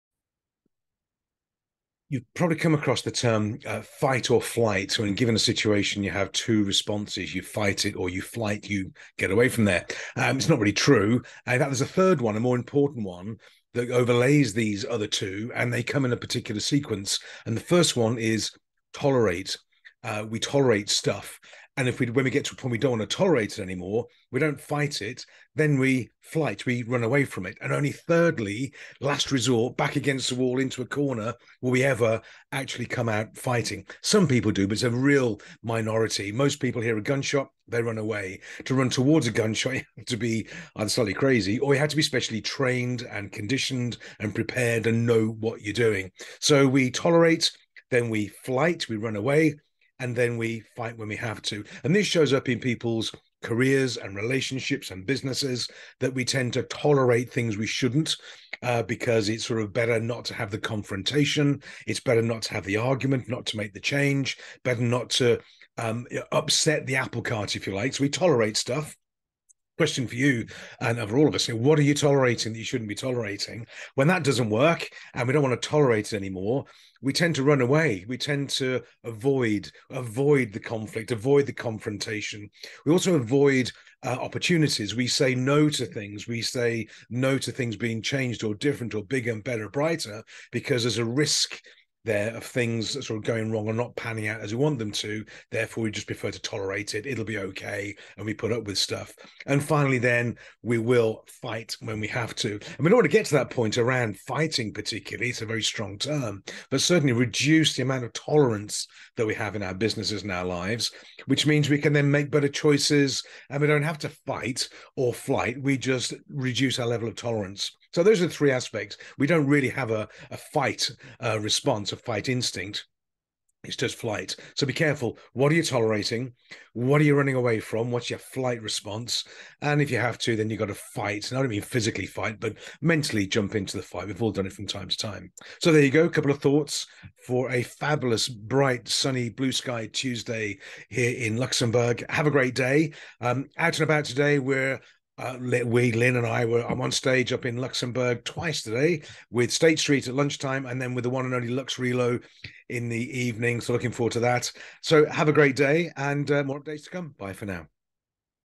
Fight, Flight or Tolerate
We have a third more powerful response to pressure and stress - Tolerate...

Most people don't have a Fight response - they Tolerate too much then when it gets bad enough they Flight, or run and hide...

Fighting is a last resort for too many...

When we agree to Tolerate less we can be more confident, assertive, and powerful. We don't need to run and hide and we never need to fight either...